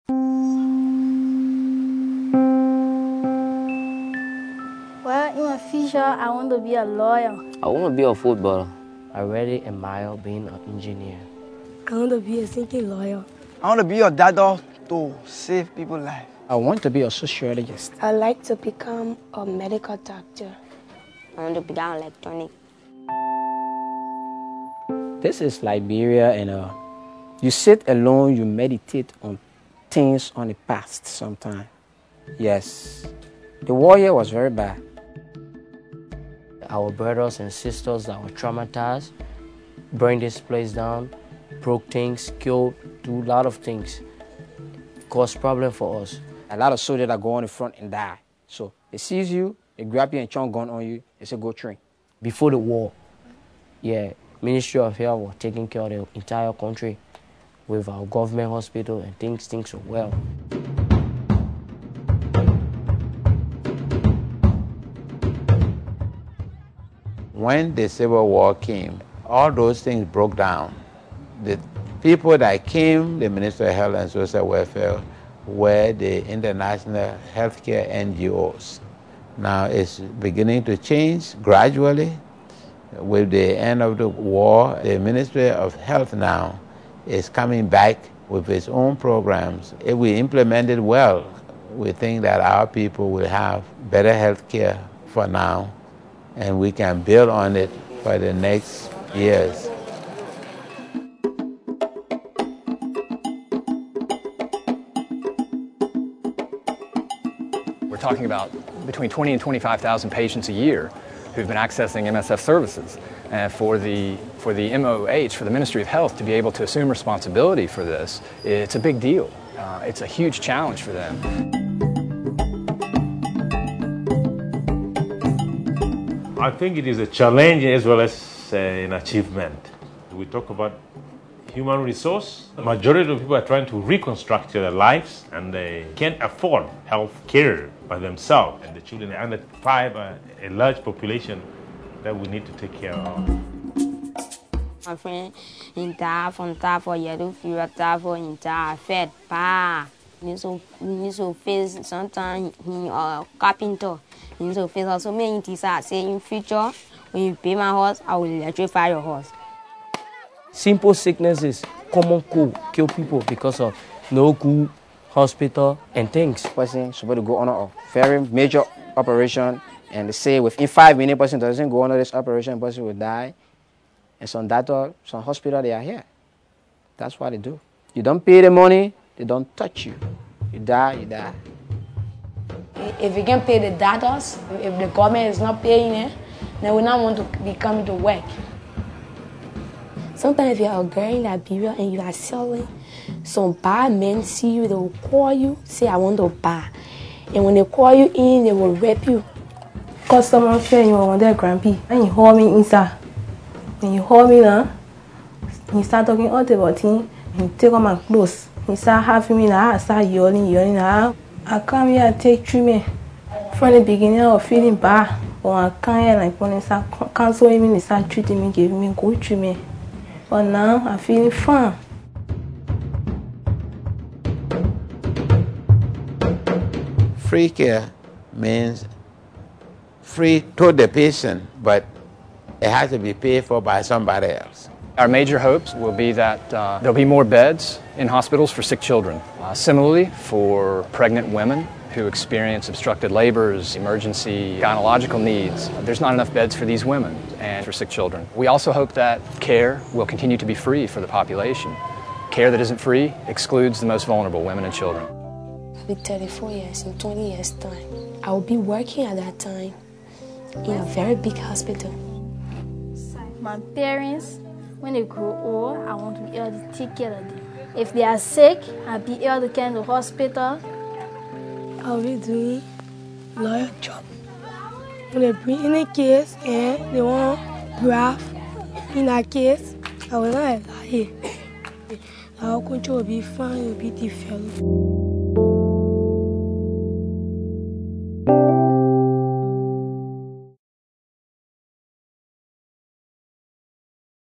Liberia Healthcare 20 Years
After 20 years of emergency aid, Médecins Sans Frontières reduces activities in Liberia, handing its hospital services over to the Liberian Ministry of Health and Social Welfare - a sign of the health systems progress just seven years after the end of 14 years of brutal civil war. As Liberia reconstructs and develops, many challenges lie ahead. Here the children of Liberia express worries for healthcare and share hopes for the future.